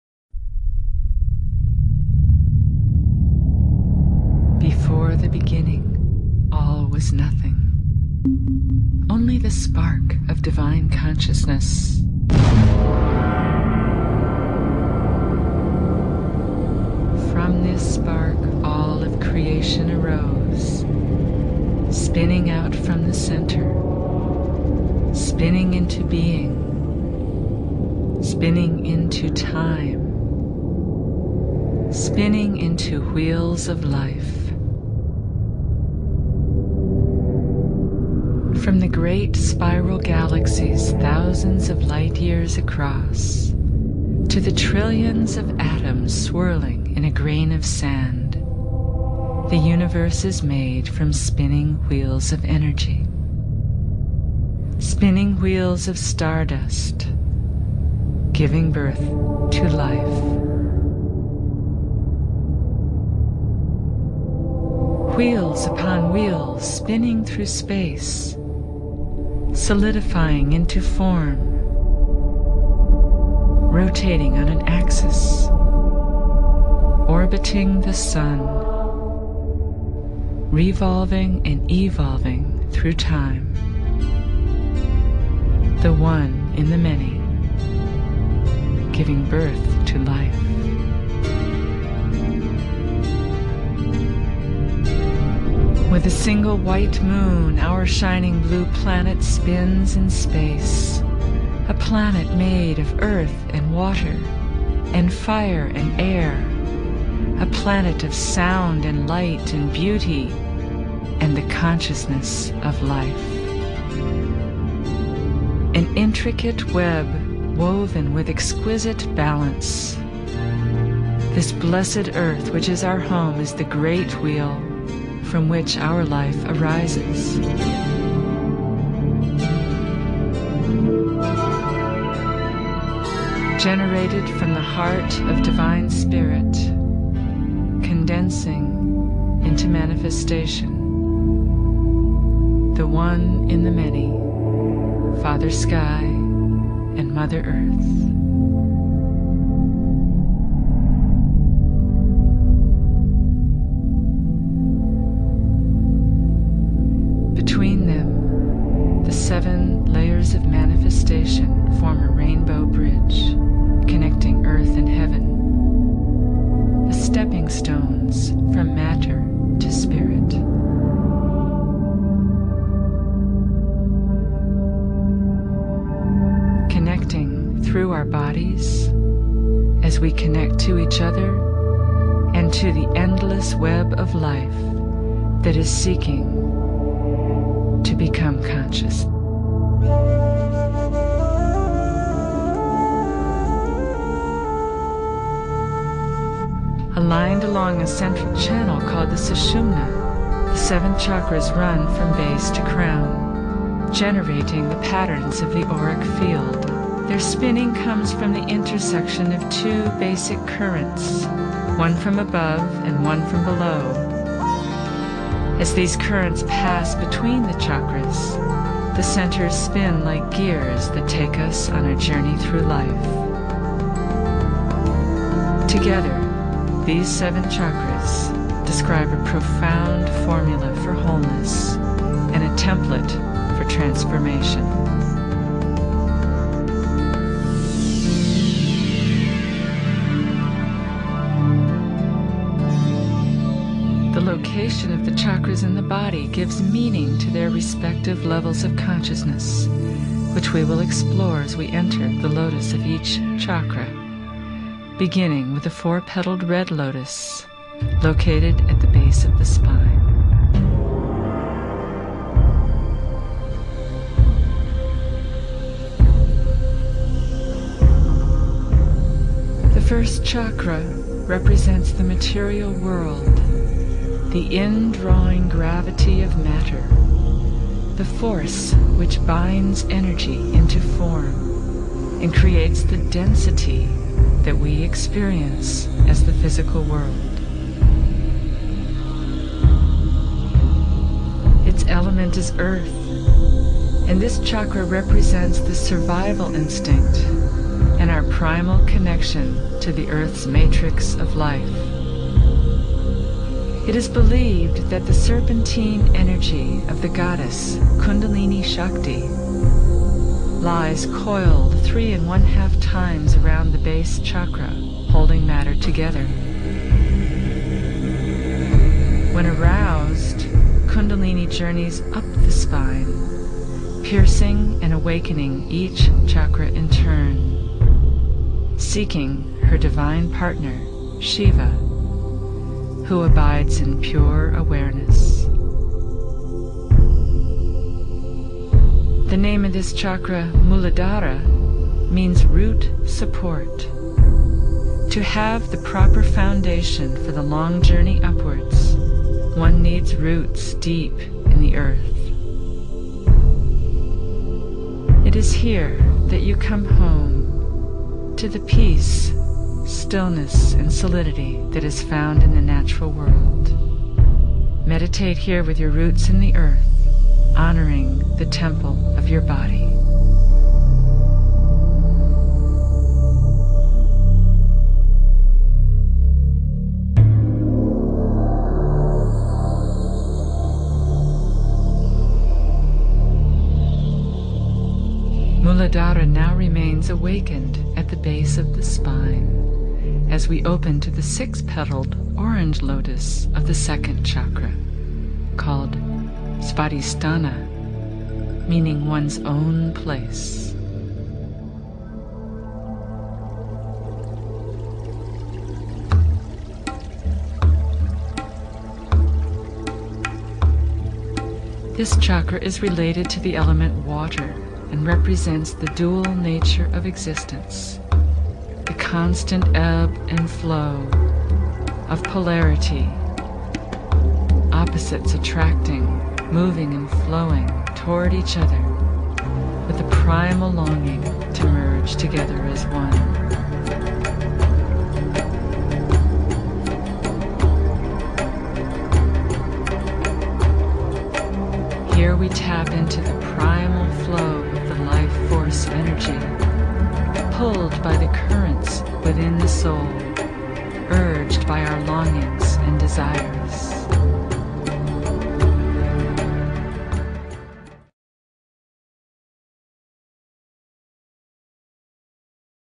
Despertar de la conciencia 7 chackras parte 1
1 - Chakra Coccígeo o Base (Muladhara).
2 - Chakra Prostático o Uterino (Svadhisthana).
3 - Chakra Umbilical (Manipura).
4 - Chakra Cardíaco (Anahata).
5 - Chakra Laríngeo (Vishuddha).
6 - Chakra Frontal (Ajna).
7 - Chakra Coronal (Sahasrara).